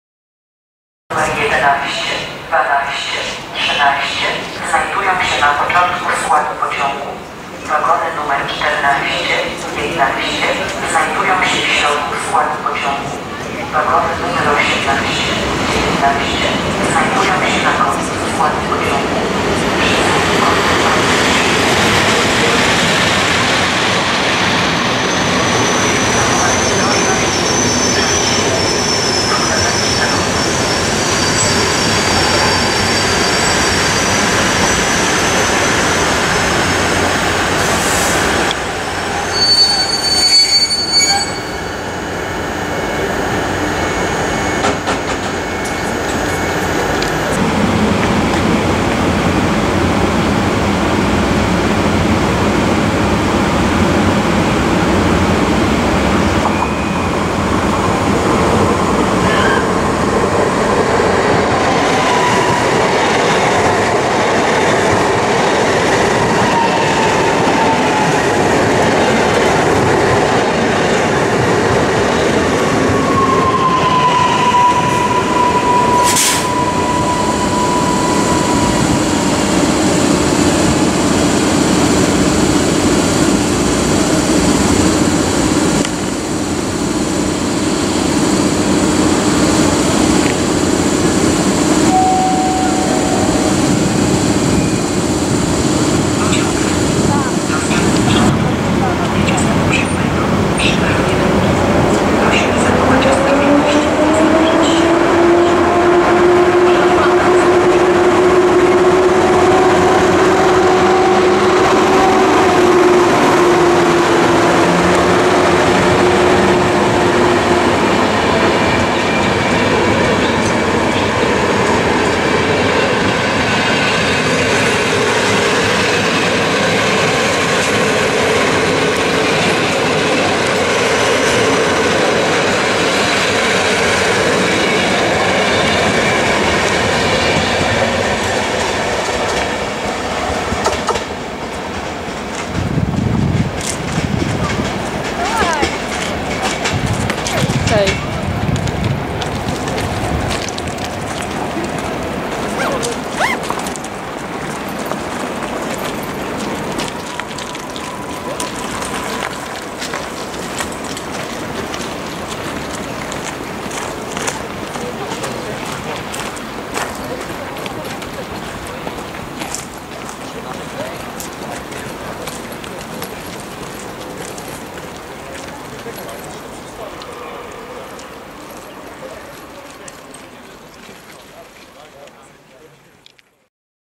EP07-442 ,, żółte czoło '' z TLK Rybak
Lokomotywa elektryczna EP07-442 która ma malowanie historyczne i charakterystyczne czoło w żółtej barwy dnia 26 lipca 2020 roku prowadził pociąg TLK Rybak relacji
Białystok - Szczecin Główny